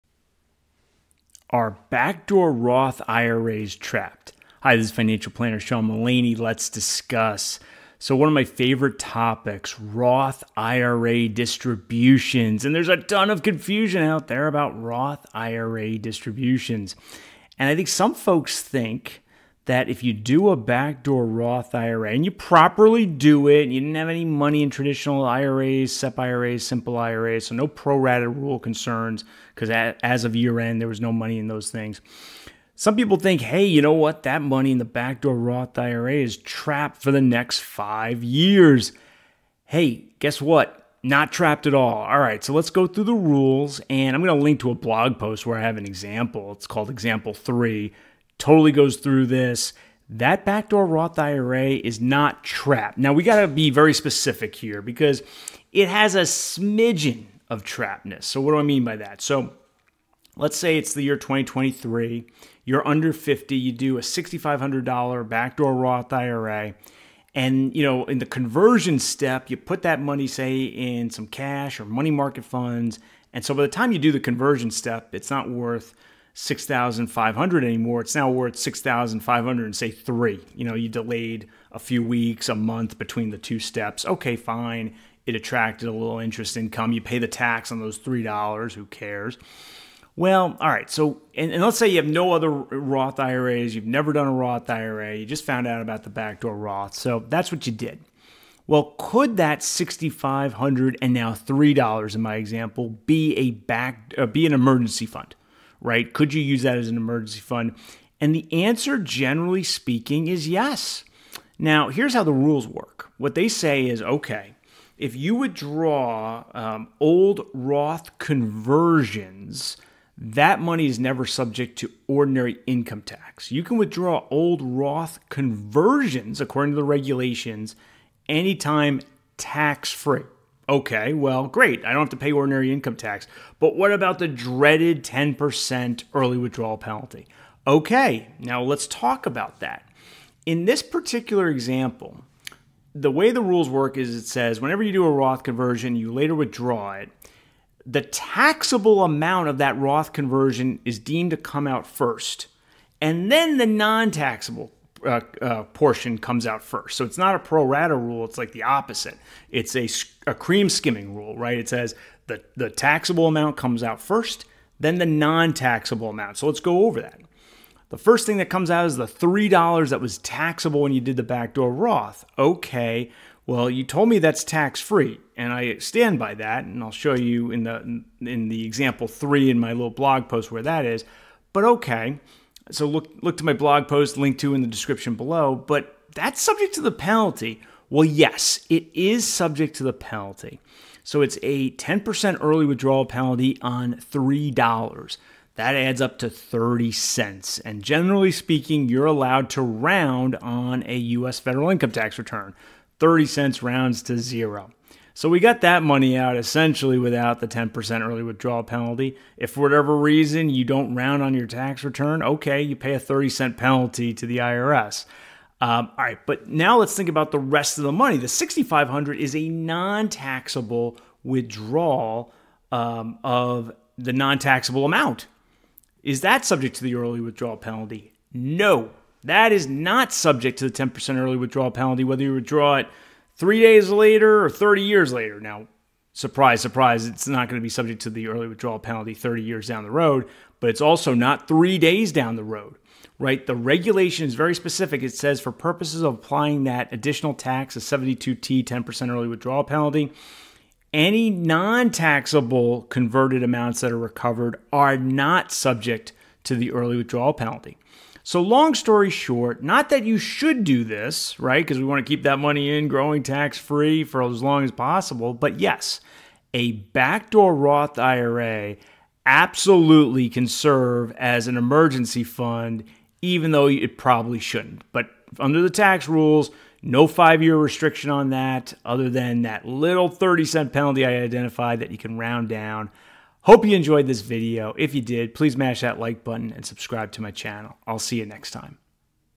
Backdoor Roth IRAs are Not Trapped!!!
People think Backdoor Roths are locked up for 5 years. Not so!

I've blogged extensively on this. See Example 5 in this blog post Technically, the withdrawal of any earnings between Steps 1 and 2 of the Backdoor Roth IRA is subject to the 10 percent early withdrawal penalty if withdrawn prior to BOTH (i) turning age 59 1/2 and (ii) 5 tax years since the conversion, but considering the amount of earnings might be zero, and is usually less than 5 dollars, the penalty is, practically speaking, very likely to be $0.

At 02:59 I said "Example 3" when I meant "Example 5." D'oh!